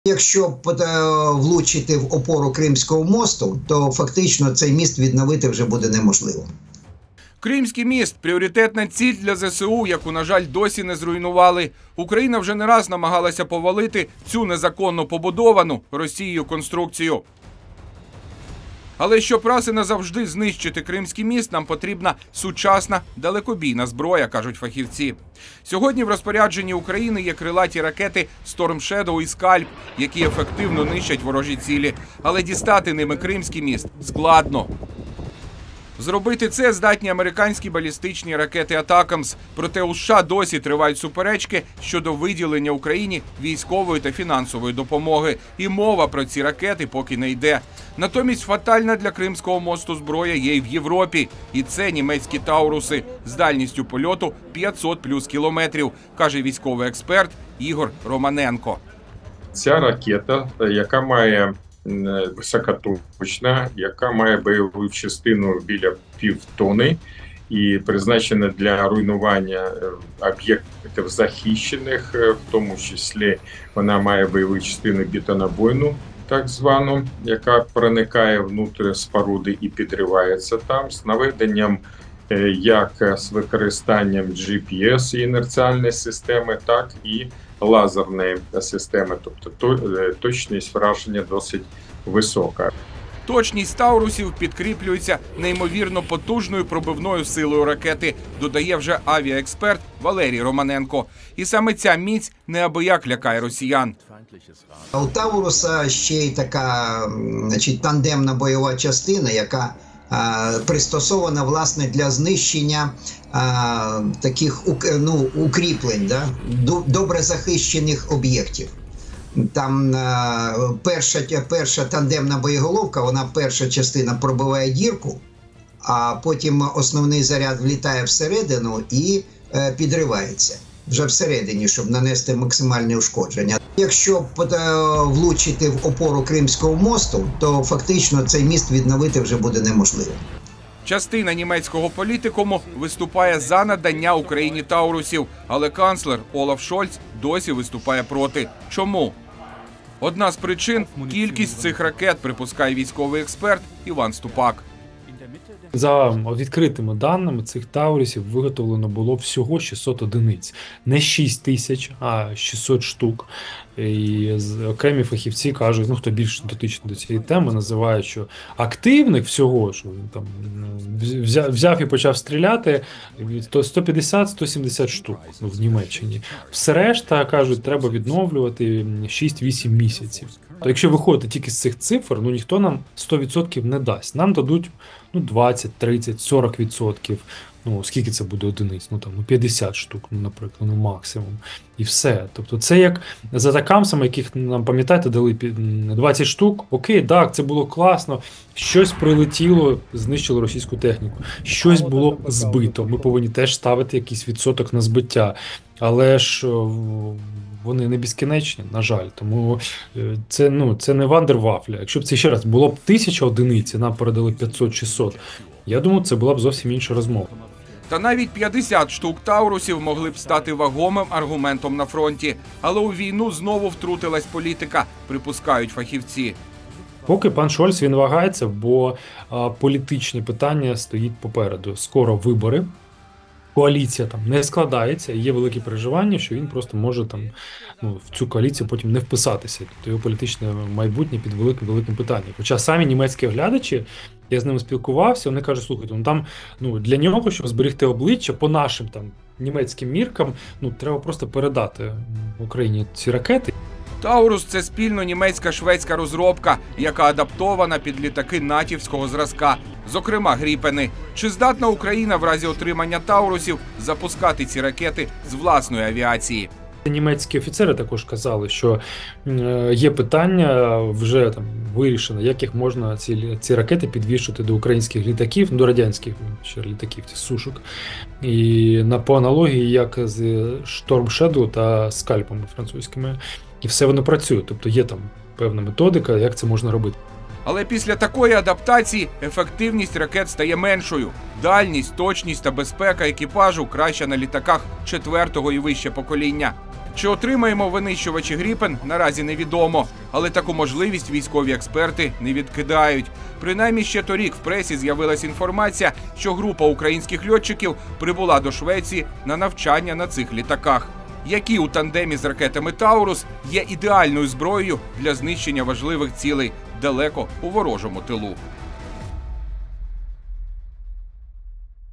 Три з чотирьох Taurus - неробочі!
В чому полягає перевага ракет Taurus над Storm Shadow та SCALP? Та коли їх може отримати Україна?

Кримський міст - пріоритетна ціль для ЗСУ, яку на жаль, досі не зруйнували. Україна вже не раз намагалася повалити цю незаконно побудовану росією конструкцію.

👉Читайте всі закриті статті розділу НВ Преміум.